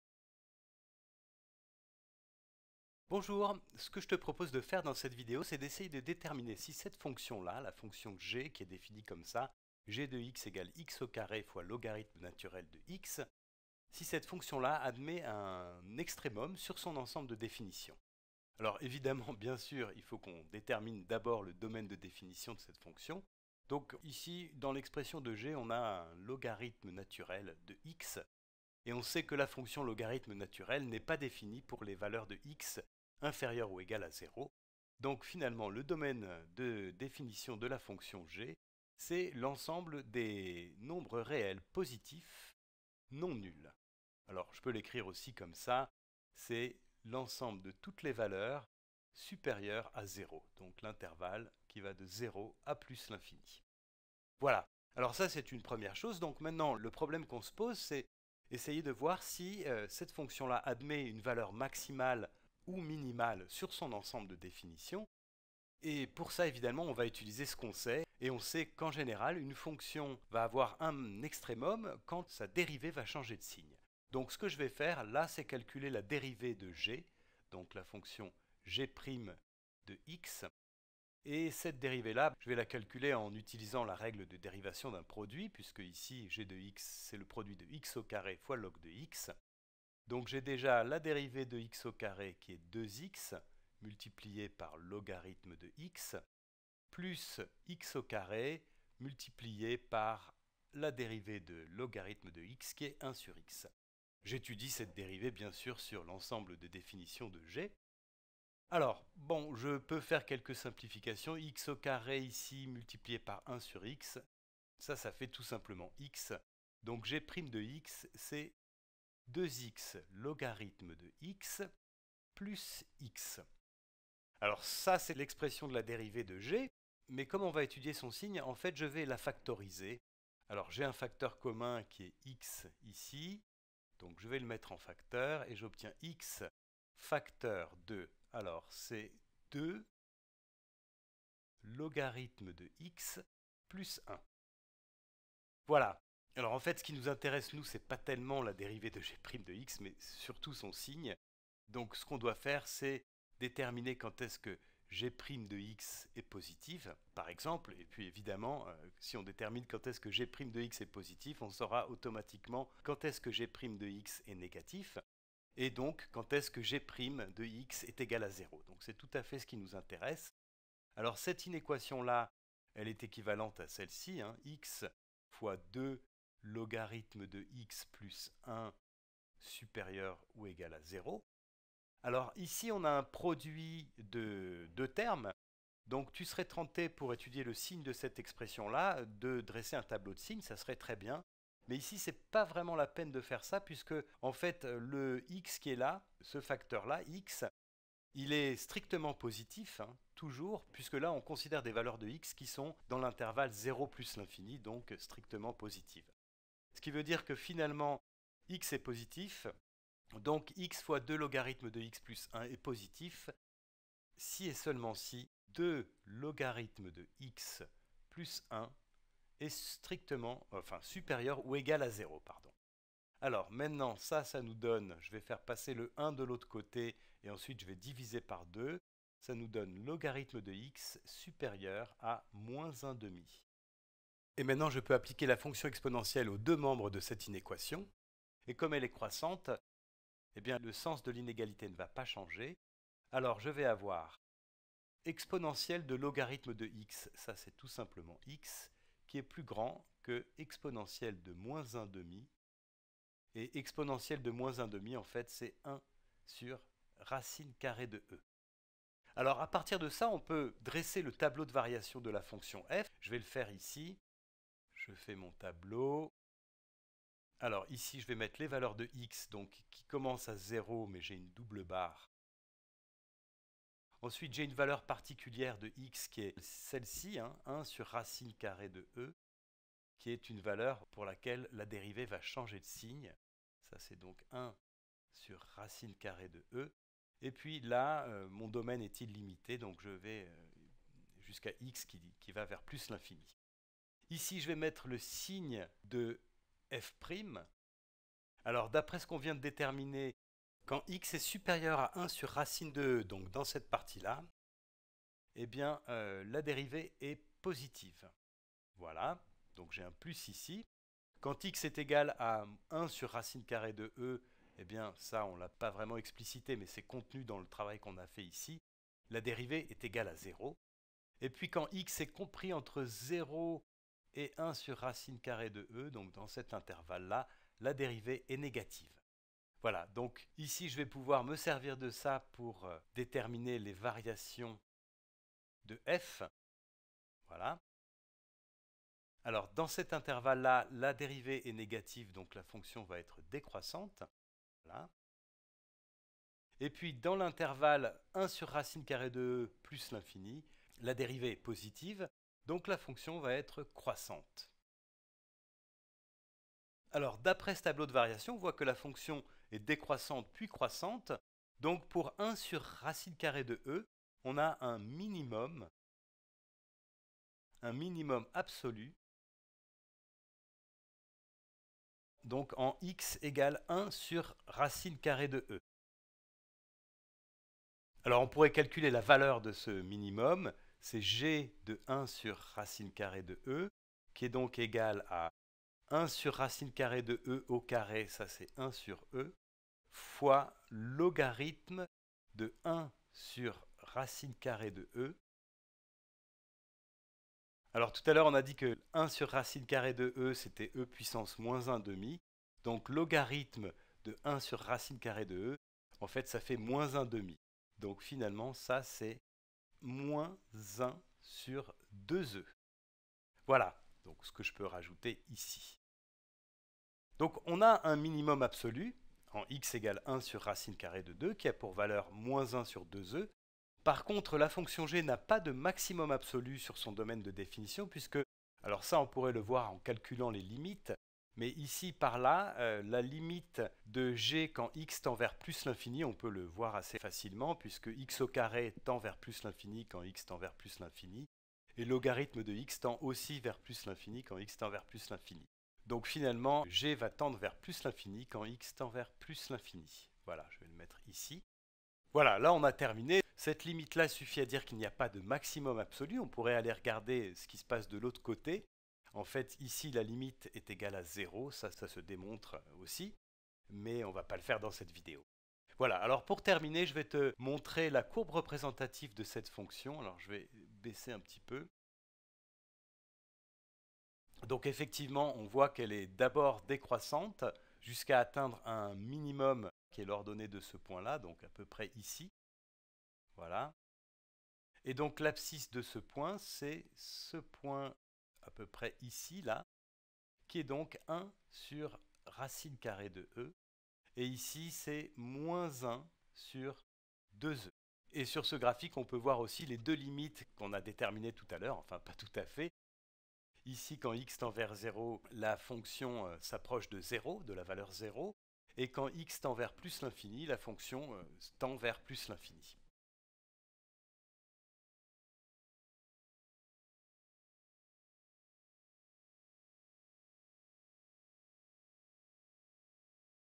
Maximum ou minimum absolu d'une fonction sur son domaine de définition
Les extremums absolus sur son ensemble de définition  de la fonction définie par g(x)=x&sup2;ln(x).